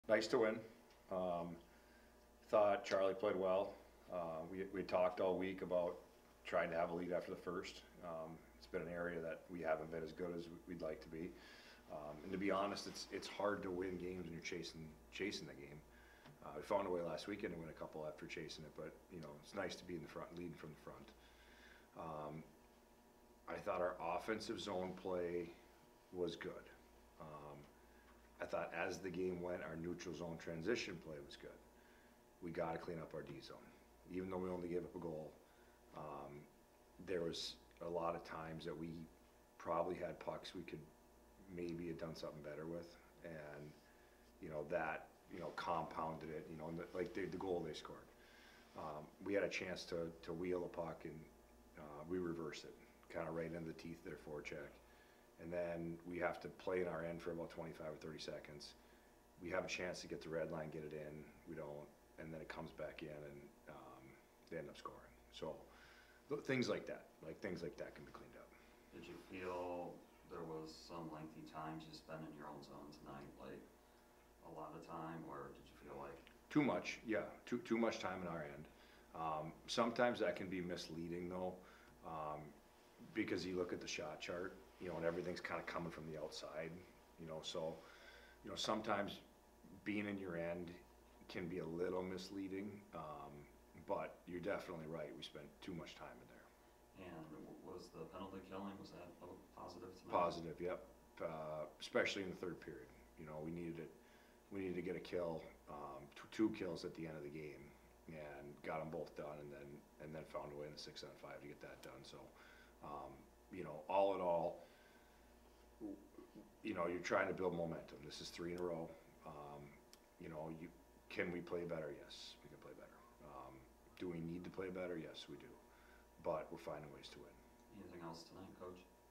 Grant Potulny Postgame Interview - November 17, 2023
Grant Potulny recaps NMU's 3-1 win over Alaska on Friday